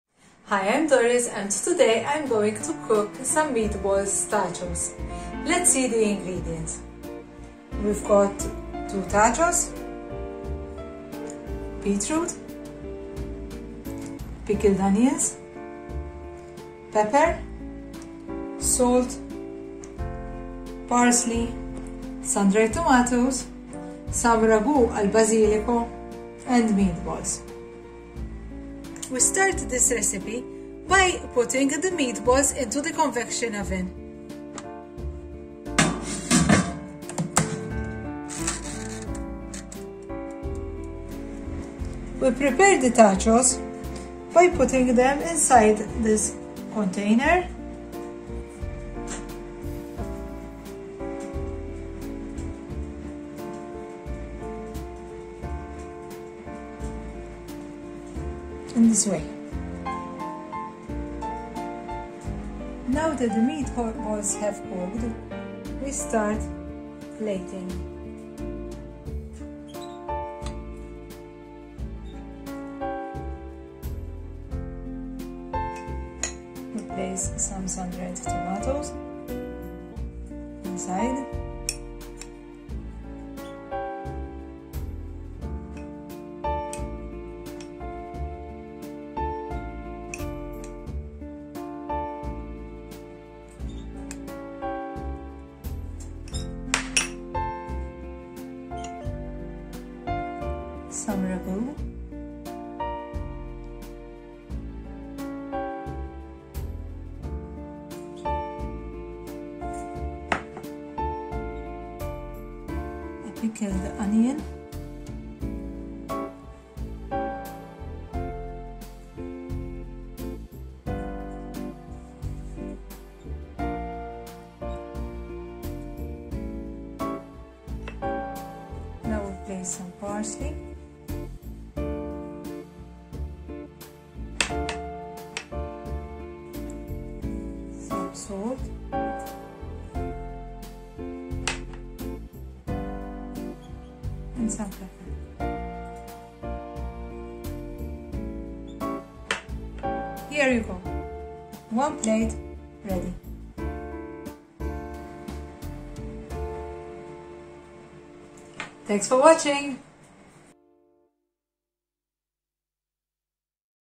FLOWER MEATBALLS
FLOWER MEATBALLS  Serves 2 persons
Total Calories 267 per serving

Ingredients
2 Tacos
Parsley
Salt
Pepper
Beetroot
Pickled Onions
Sundried Tomatoes
Meat Balls
Ragu Al Basilico